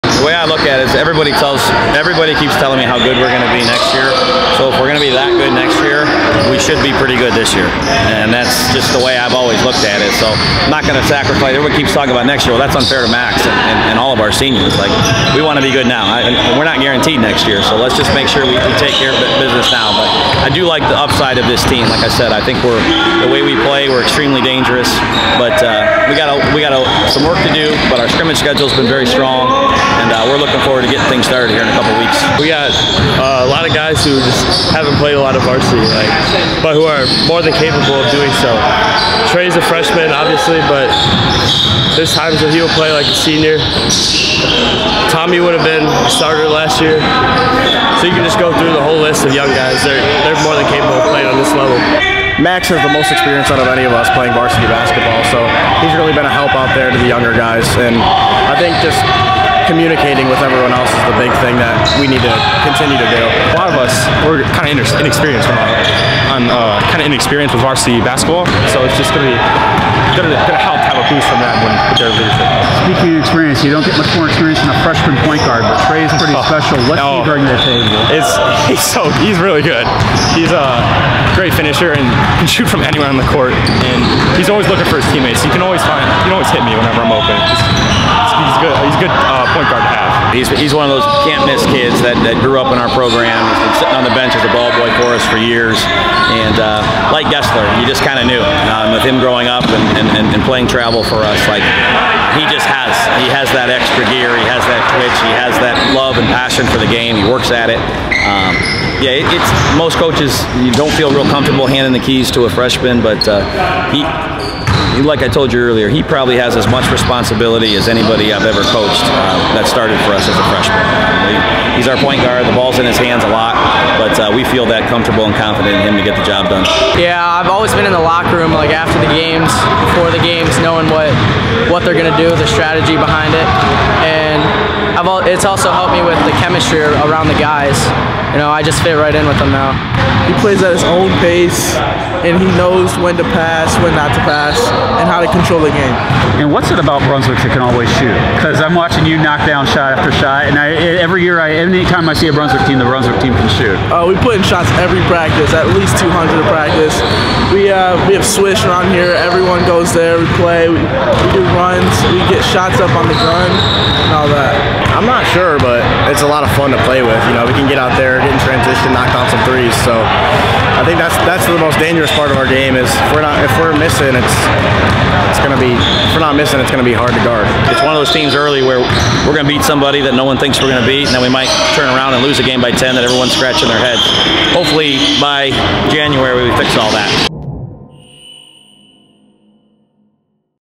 Brunswick's upside starts now
With just two experienced seniors, Brunswick welcomes back a healthy junior forward in Tommy Morgan and freshman point guard Trey Drexler to a team full of potential. Will it start now with the 2022-23 high school basketball season?